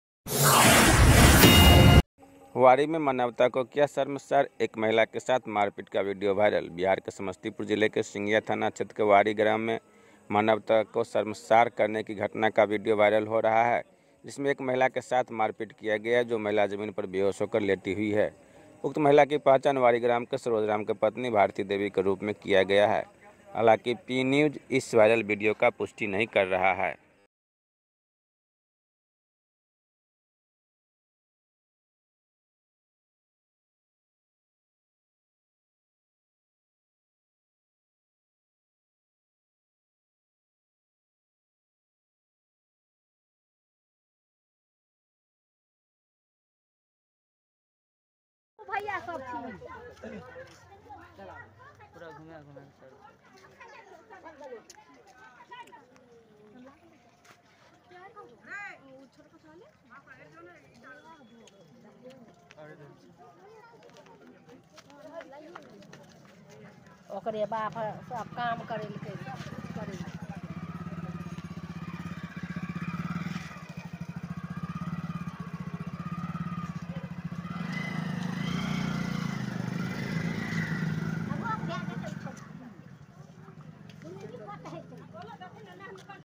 वारी में मानवता को किया शर्मशार एक महिला के साथ मारपीट का वीडियो वायरल
बिहार के समस्तीपुर जिले के सिंघिया थाना क्षेत्र के वारी ग्राम में मानवता को शर्मशार करने की घटना का वीडीओ वायरल हो रहा है जिसमे एक महिला के साथ मारपीट किया गया है जो महिला जमीन पर बेहोश होकर लेटी हुई है।उक्त महिला की पहचान वारी ग्राम के सरोज राम के पत्नी भारती देवी के रूप में किया गया है हालांकि पी न्यूज़ इस वायरल वीडियो का पुष्टि नही कर रहा है Breakingnews #Breaking_news Live LiveNews latestnews PNews PNewsLive News aaj_ki_taja_khabar #aaj_ki_taja_khabar_Live